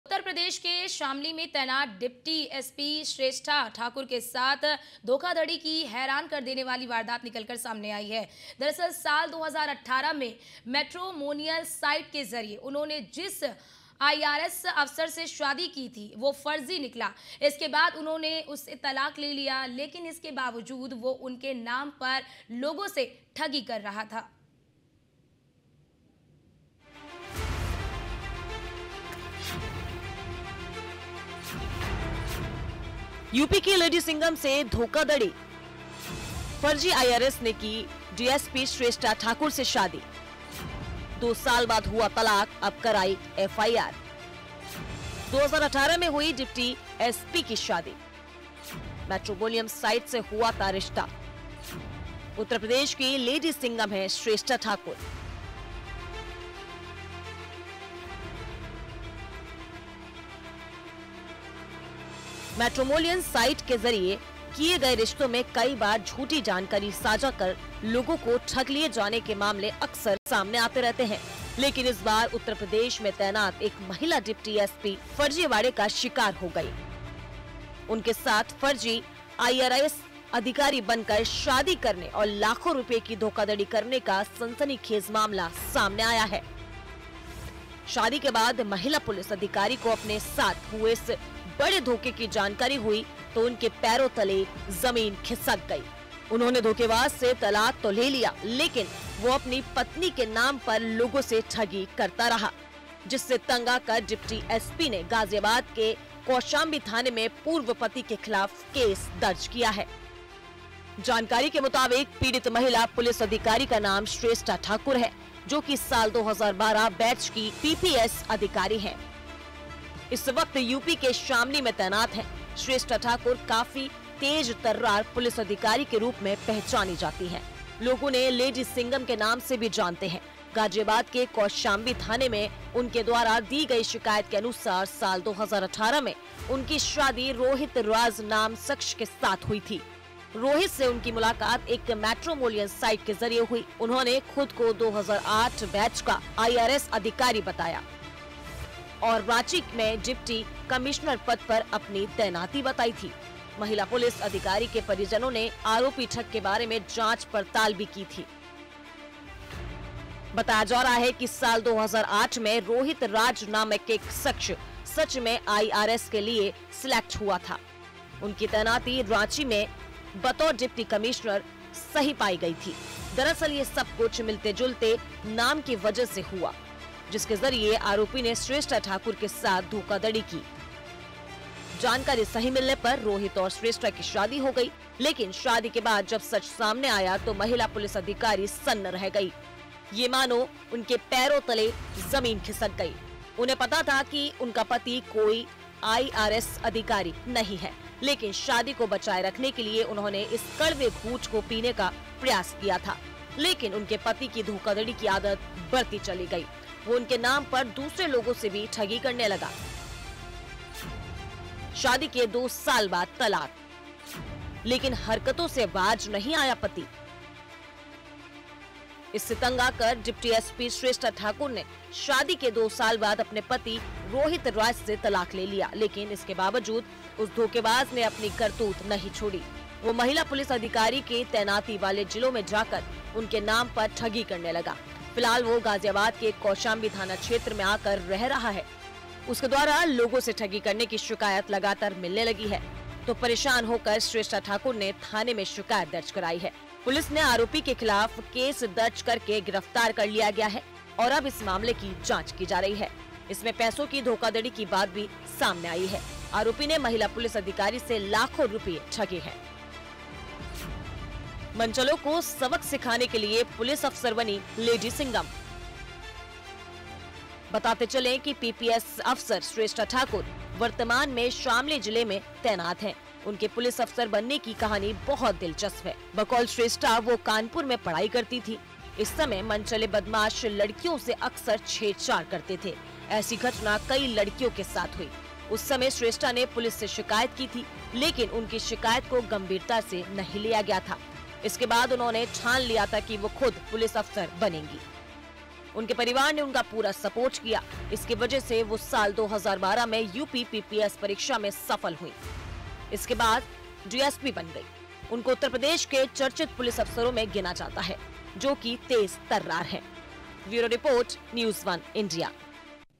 DSP Shrestha Thakur Case : Up की लेडी सिंघम से धोखाधड़ी... | Shrestha Thakur VS Bjp Leaders
Airtel - 350,
Netvision - 219,
RCN - 403,
UCN-223
Alisha Digital- 285
HDS- 127

We are also available on:
MX Player
JIO TV